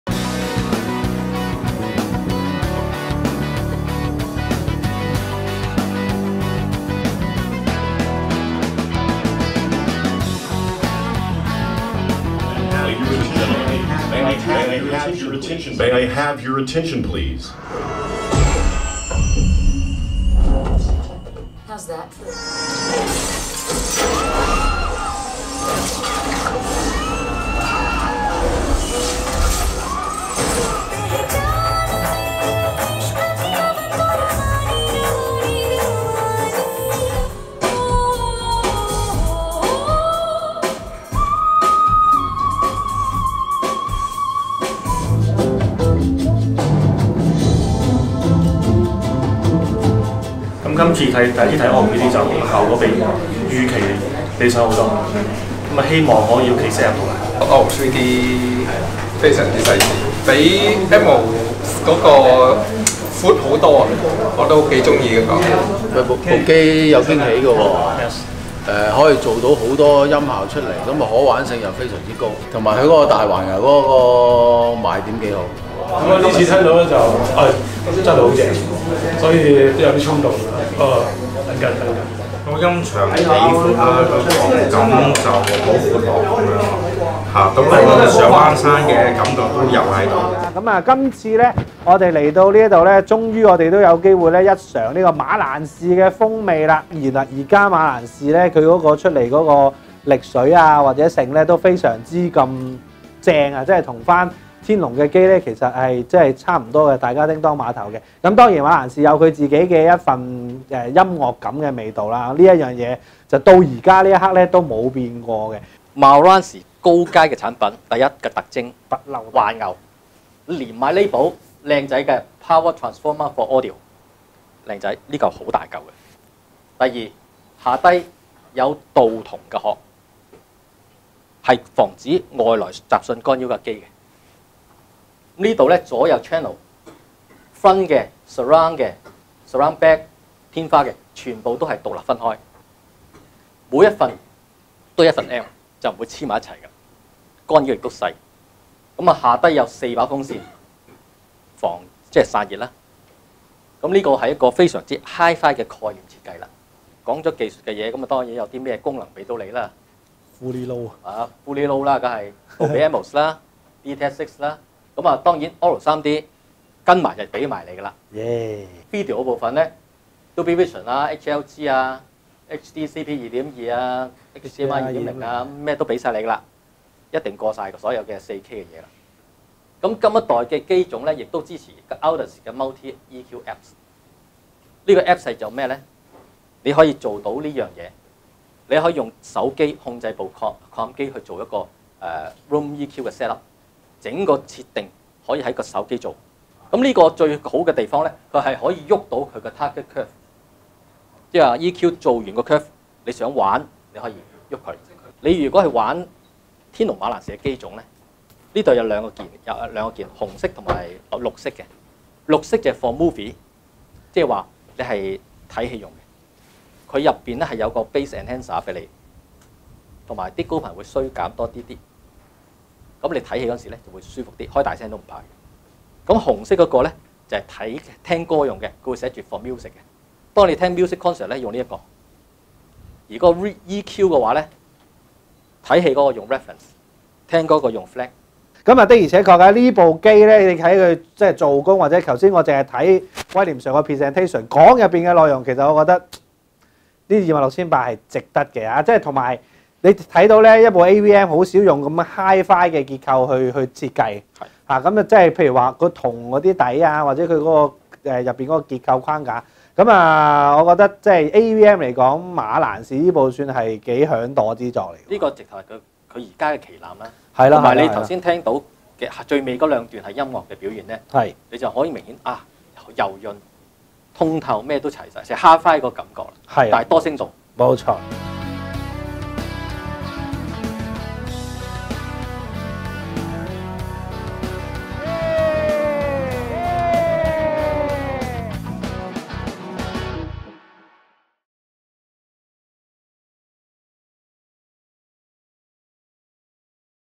【活動報導】Marantz SR8012 旗艦合併擴音機搶先試
為讓大家自己親身試過 Marantz 最新旗艦 SR8012，我地最近就為這部新作舉行了一連兩場體驗活動，親身試真呢部高規格型號力水可去到邊。